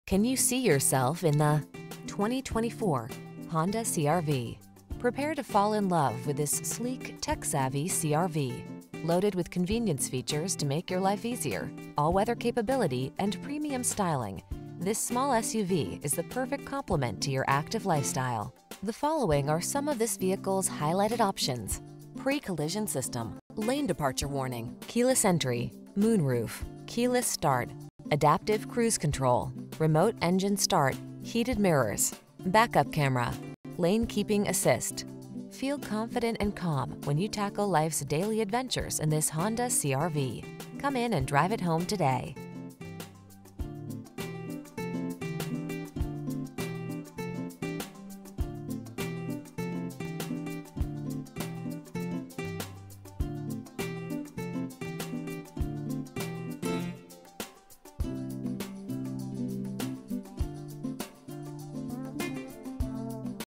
2024 Honda CR-V Hybrid Marietta, Atlanta, Roswell, Woodstock, Kennesaw, GA 497048
Platinum White Pearl New 2024 Honda CR-V Hybrid Sport available in Marietta, Georgia at Ed Voyles Honda. Servicing the Atlanta, Roswell, Woodstock, and Kennesaw, GA area.